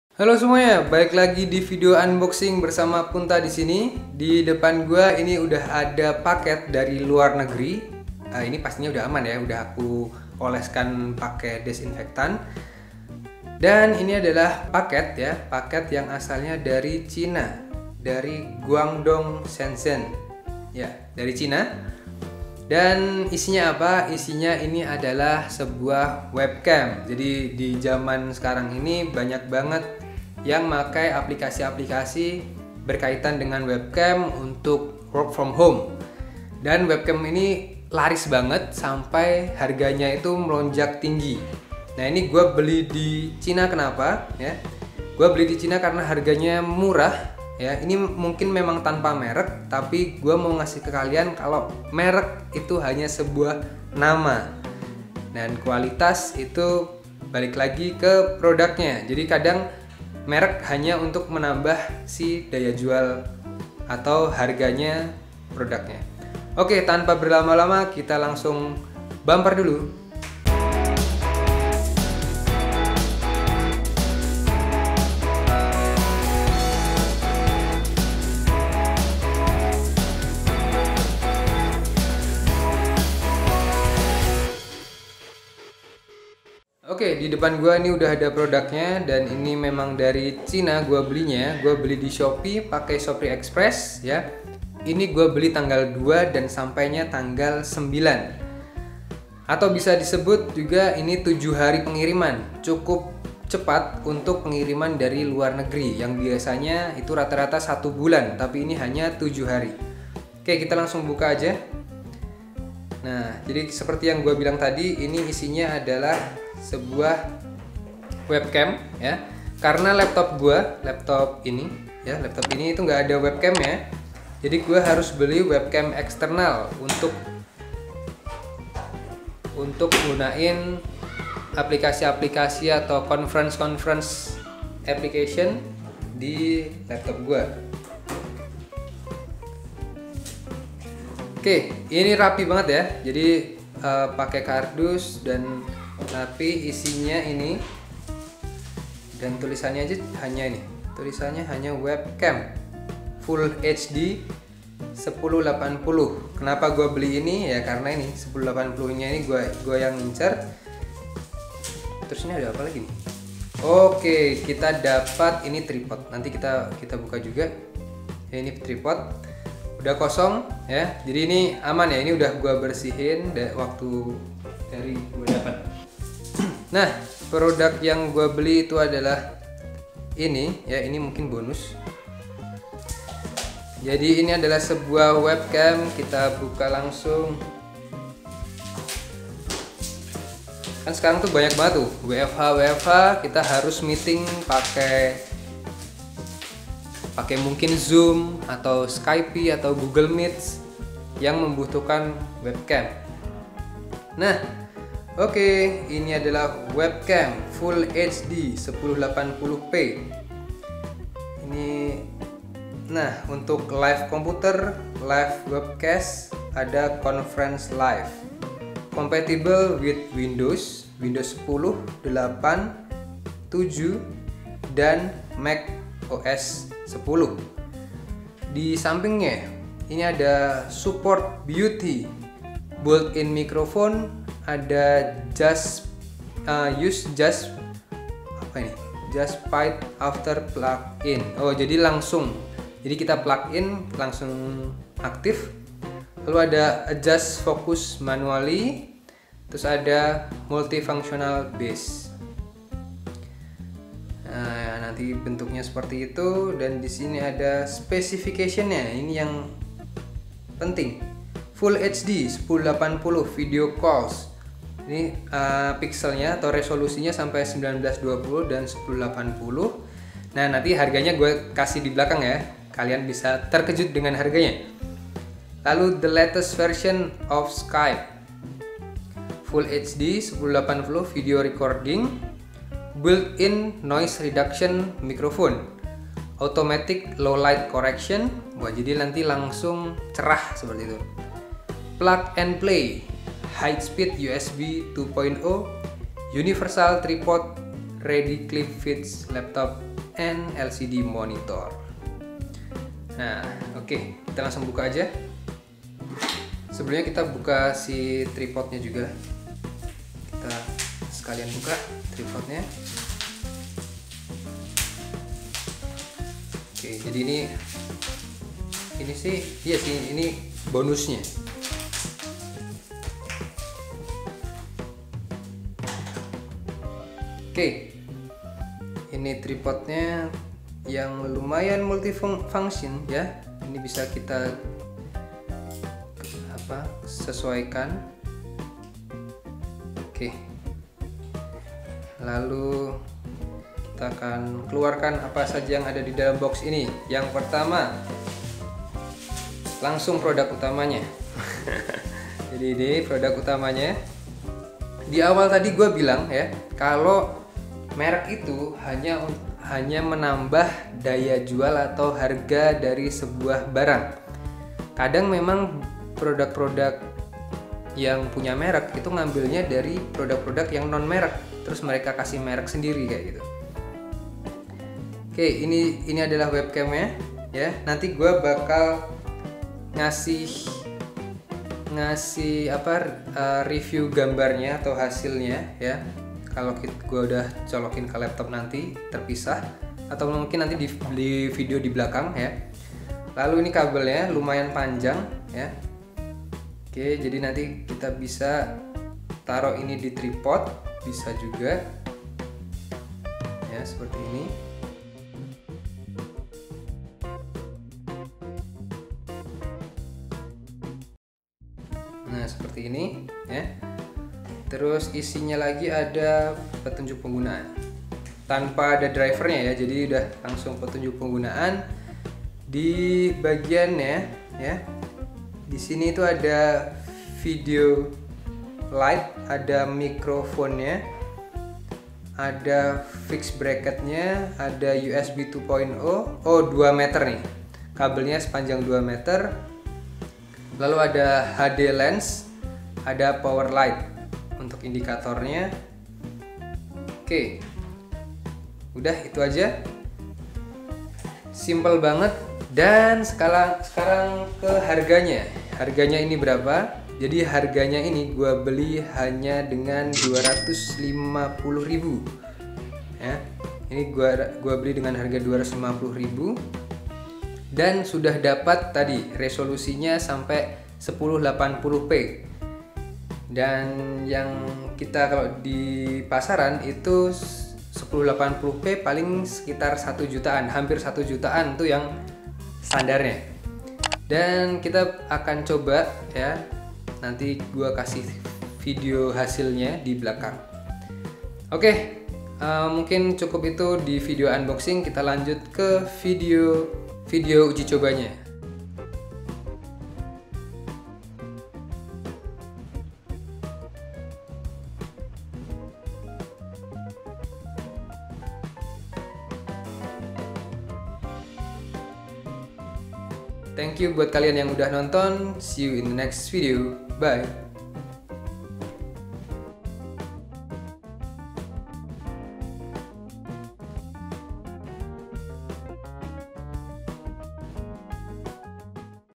Webcam Youtuber Pemula Bisa Zoom Google Meet Murah | Unboxing Review
Puas banget beli item ini, webcam murah tapi udah Full HD 1080p.
Sasaran sahabat kere hore nih! Murah dan kualitas premium!